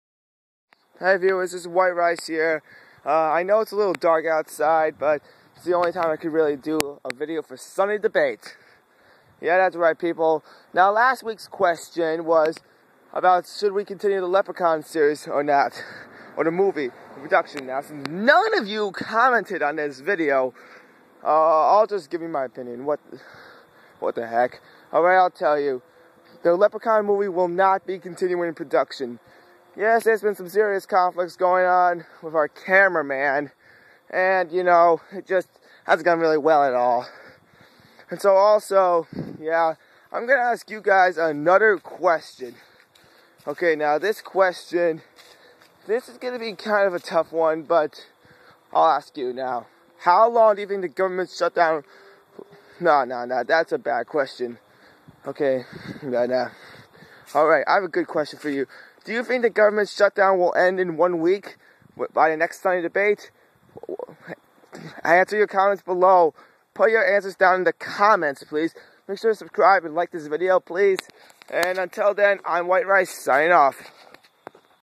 Sunday Debate 4
White Rices asks another question for the viewers to answer.